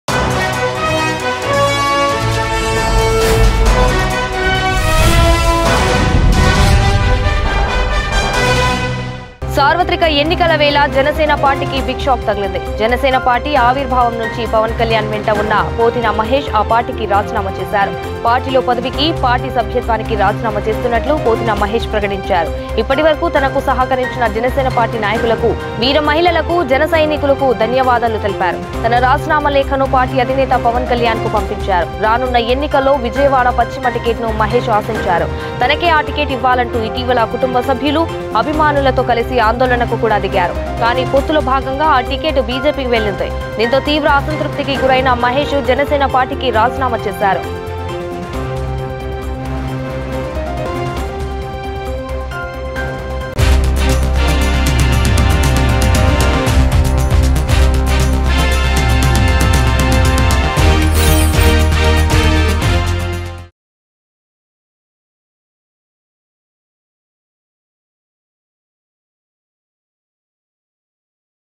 Janasena Leader Pothina Mahesh Resigns From Janasena Party | Pawan Kalyan | AP Politics | Mango News
Janasena Leader Pothina Mahesh Resignation Janasena Party. Following the allocation of Vijayawada west seat to NDA candidate and former Union Minister Sujana Chowdary Pothina Venkata Mahesh has resigned from the party and all the positions he was holding. Andhra Pradesh Elections To Be Held On May 13th, 2024.